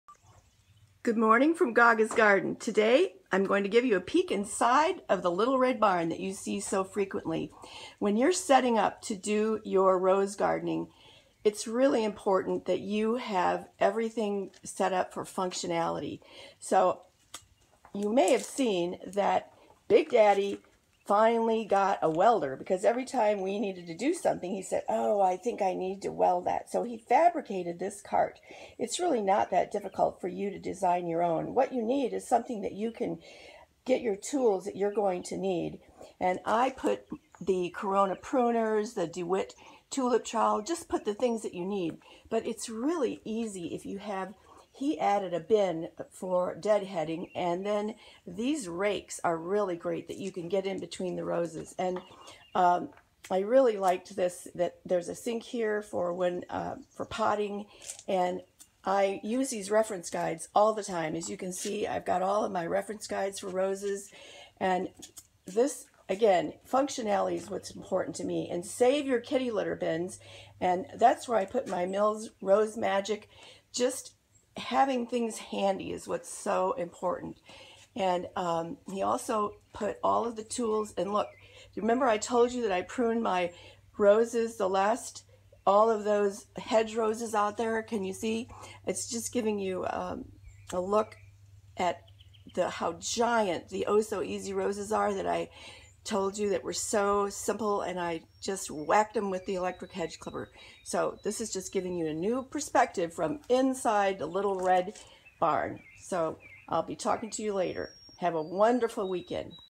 Inside Gaga's Rose Garden Little Red Barn
The little red barn is a symbol for country rose gardening. See how you can get organized with easy gardening tips.